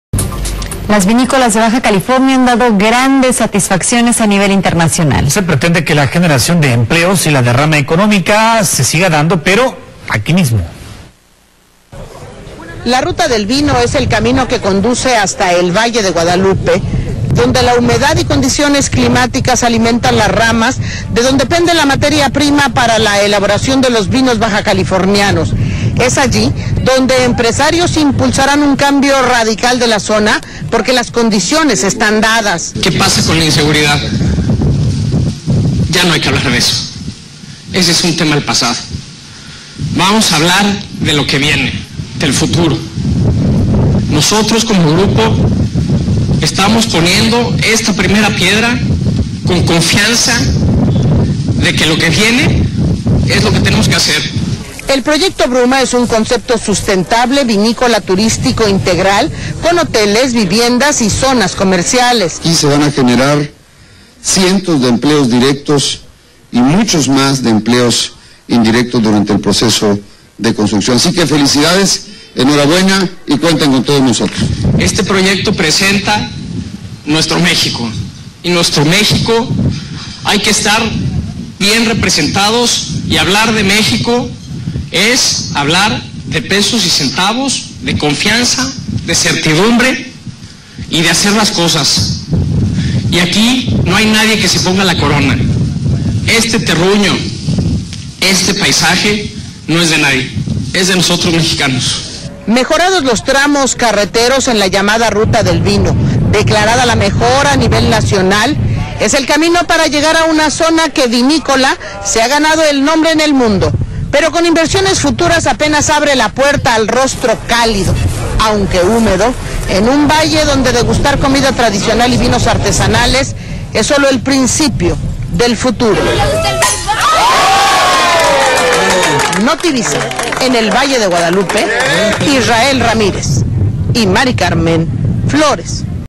Primera Piedra del Proyecto BRUMA en Notivisa Regional
La periodista Maricarmen Flores nos presenta un reporte de la ceremonia de Primera Piedra del Proyecto Turístico - Enológico "Bruma Valle de Guadalupe".